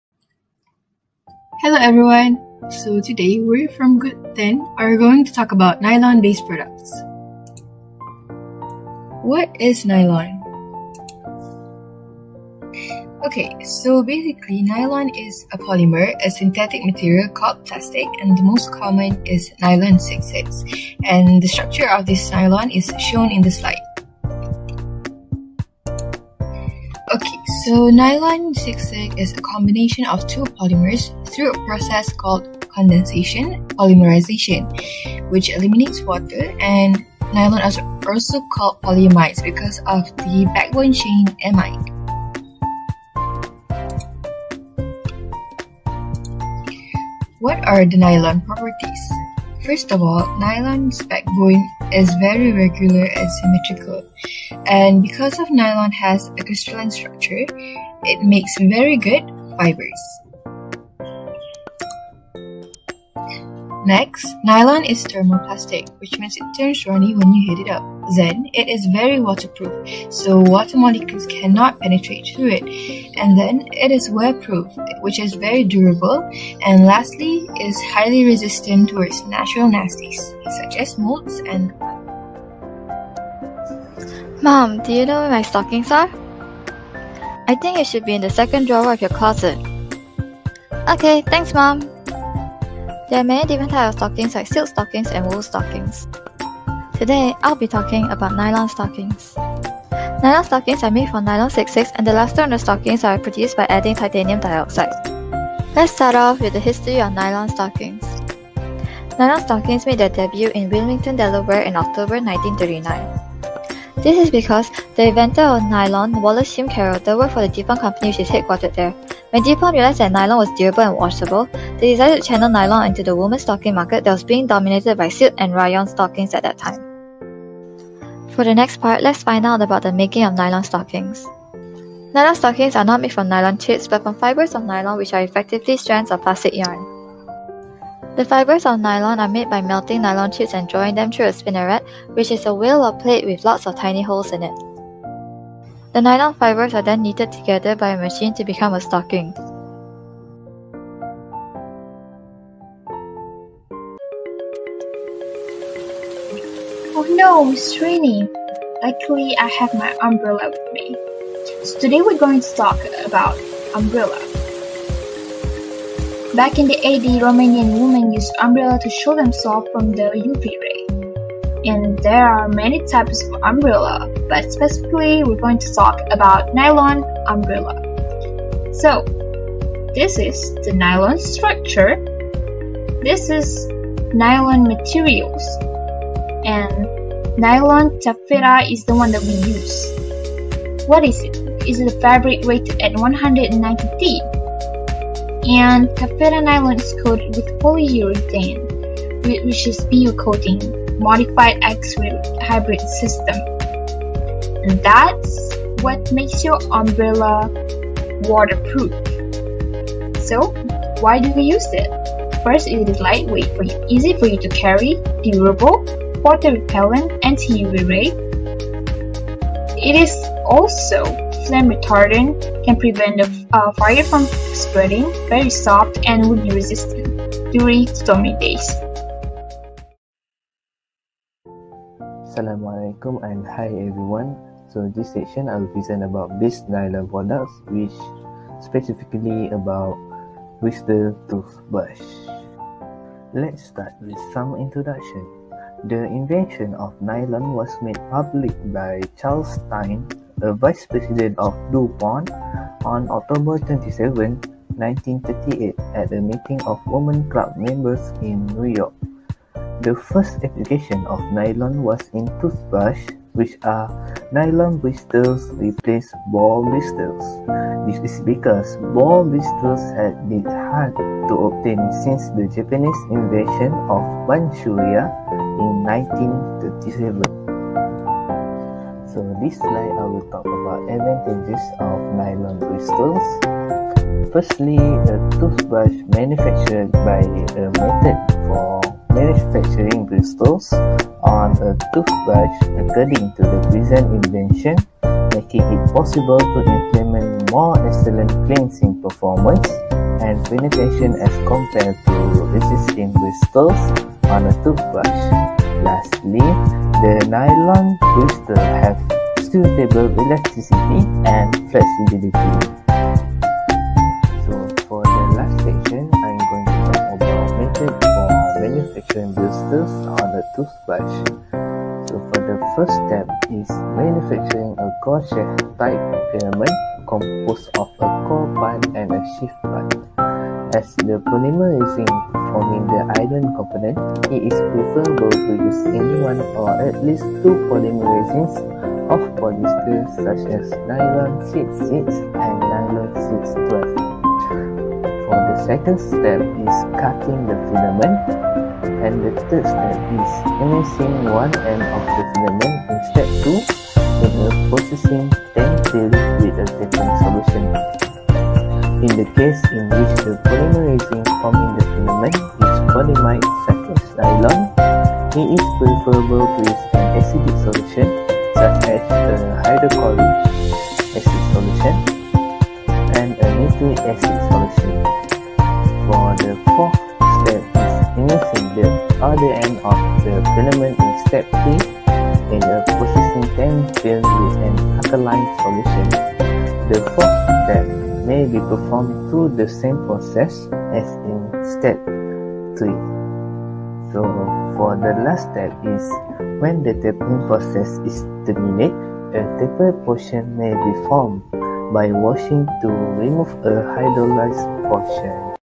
K16 - NYLON BASED PRODUCTS
Salam and hi everyone.

On 22 May 2021, a competition of "5 Minutes Video" was held by Chemistry Unit ASPER UPM entitled: Animated Video for Agricultural or Industrial Important Elements”.

This is one of the best video created by students from ASPER 16 UPM Kuliah 16.

Enjoy the video and have fun watching! 🥰